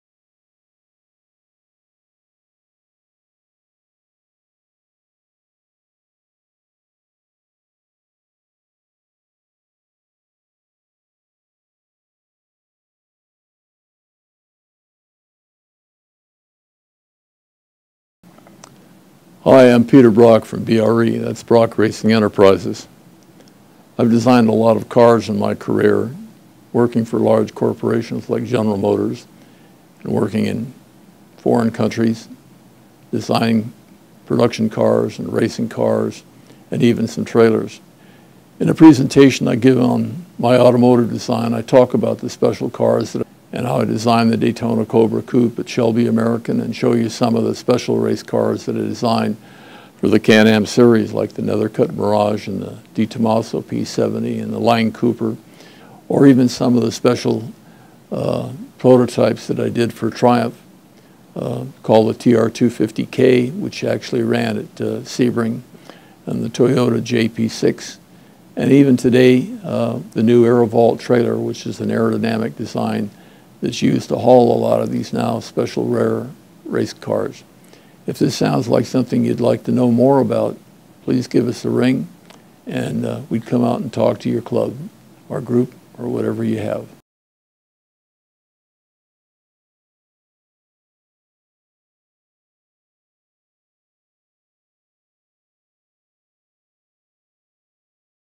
Peter Brock Speaks About: My Personal Design History
Between GM Styling, Shelby American and Peter Brock's own firm, Brock Racing Enterprises, designer Peter Brock has designed more iconic sports cars than any other American designer. In this video he talks about the amazing cars and insider stories behind these cars that he can share when he presents at your event or to your group.